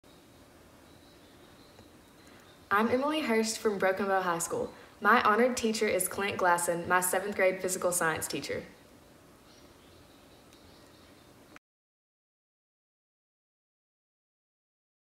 Emily Hurst Academic All-State Intro Video
For our televised Academic Awards Tribute, we asked Academic All-Staters to submit a brief video introducing themselves and an honored teacher that they had planned to invite to the Academic Awards Banquet. Each student recognizes an honored teacher who has had a profound impact on his or her life.